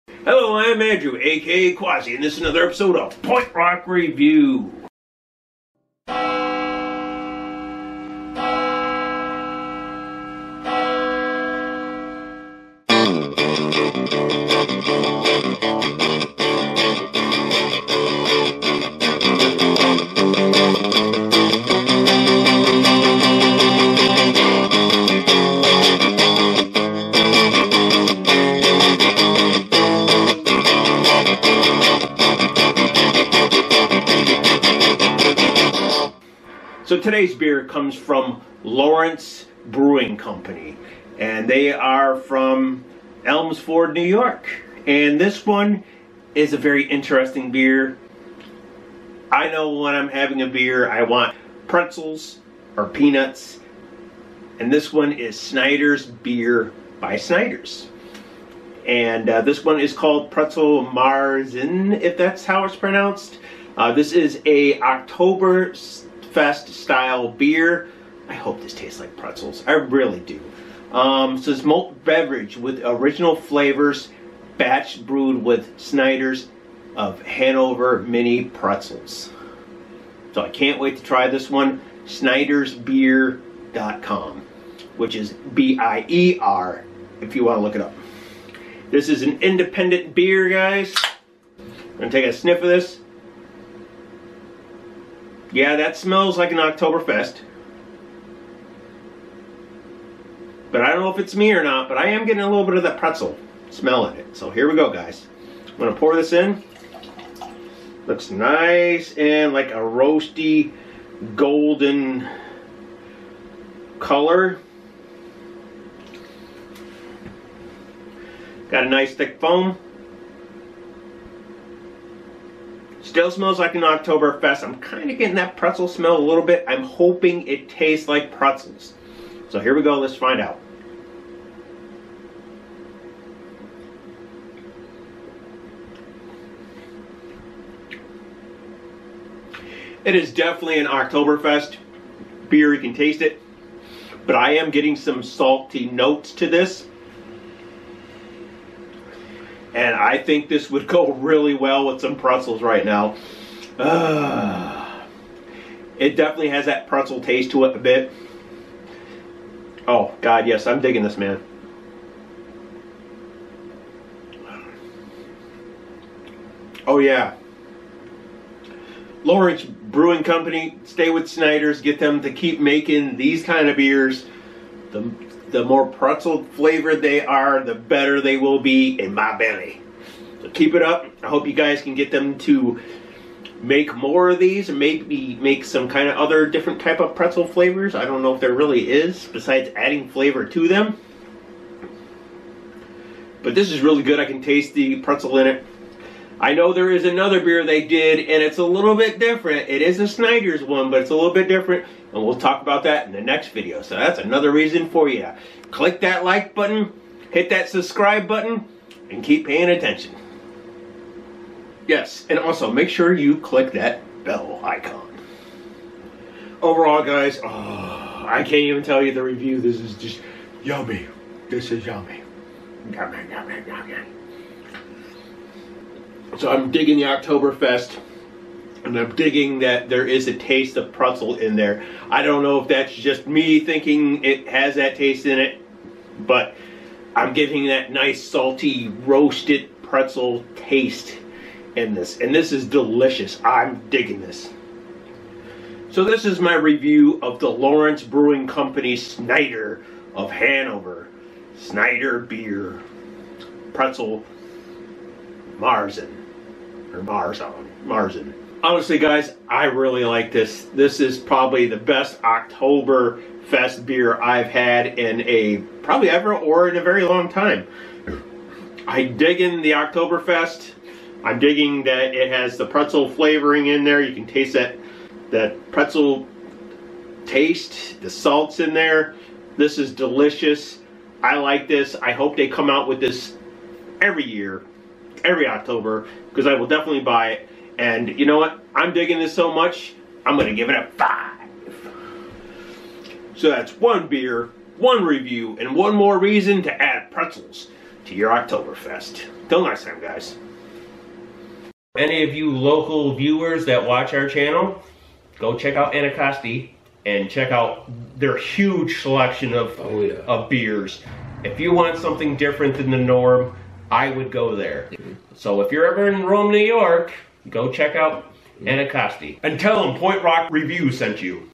Lawerence Brewing Co. Snyder Bier Pretzel Marzen
In this video, i drink and review Lawerence Brewing Co. Snyder Bier Pretzel Marzen

On this channel, Quassi from 'Quassi's Bell Tower' and BrotherBrian from 'BrotherBrianDiscoveries' taste-test beer and give ratings from 1 to 5 stars.